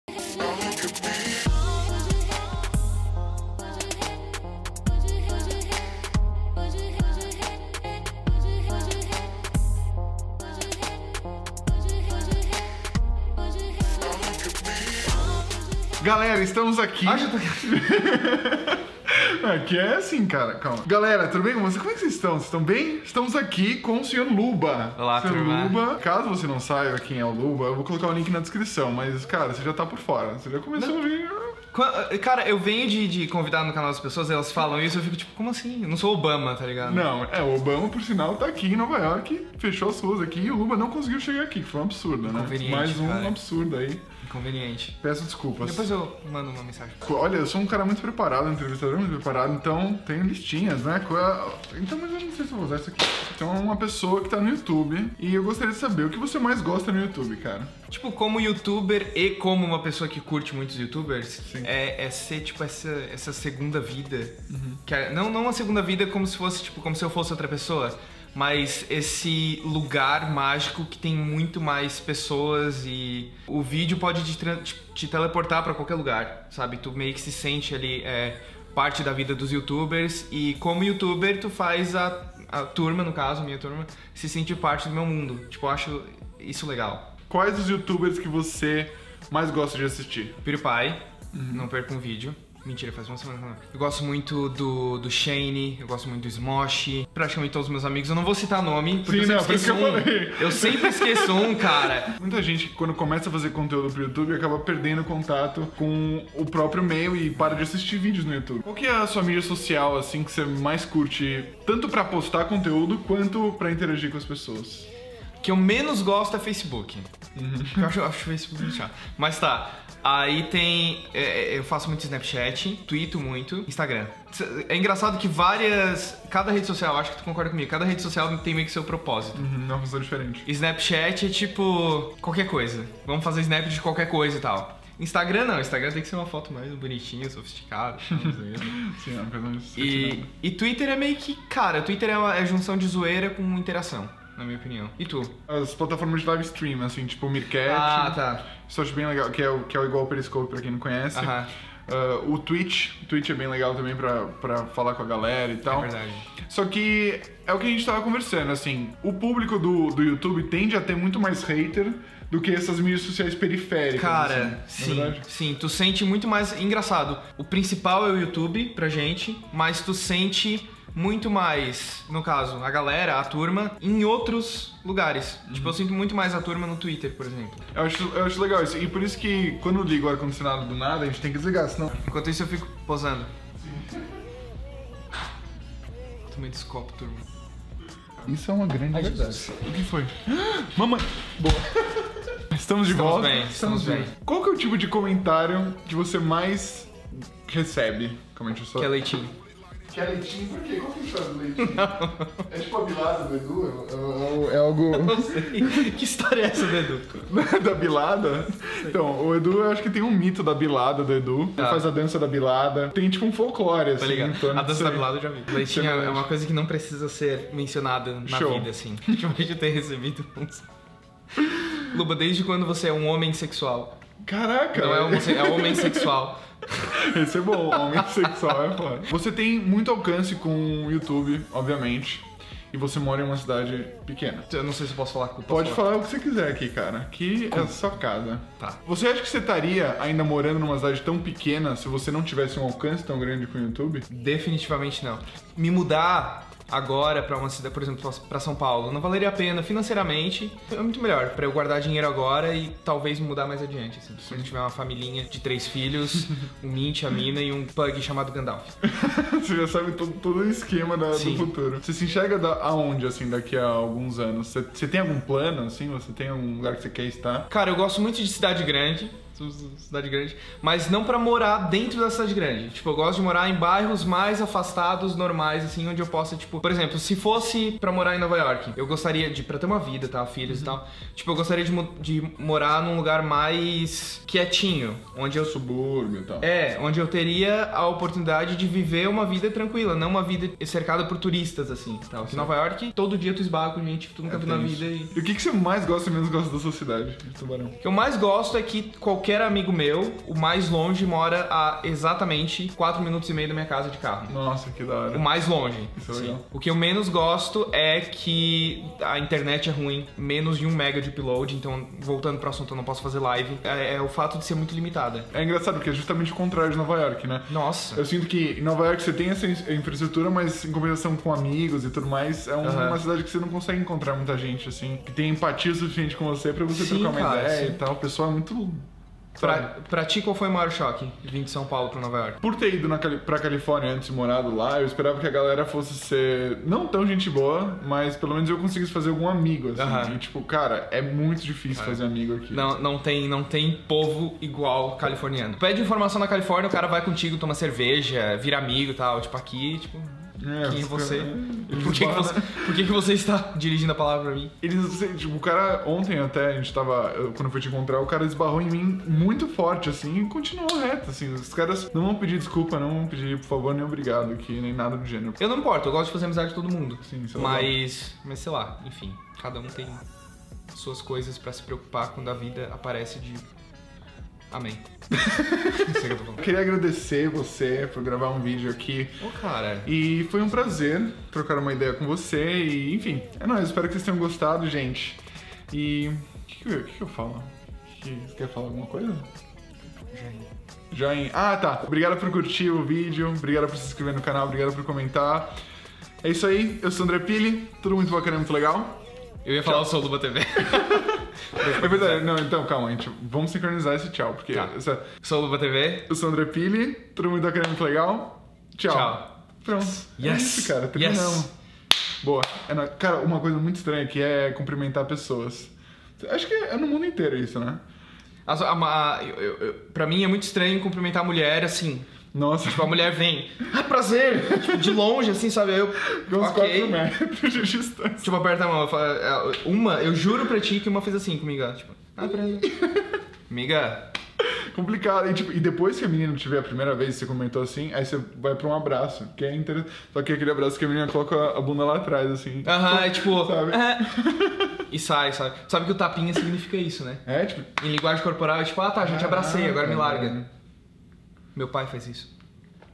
A VIDA DE YOUTUBER ft. LUBATV
Trocando ideia com o senhor Luba sobre a vida de youtuber, morar em uma cidade pequena e a sua querida turma.